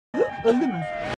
Öldü mü ?
Cem Yılmaz Doritos reklamından bir kısa kesit
Kısa Kesitler.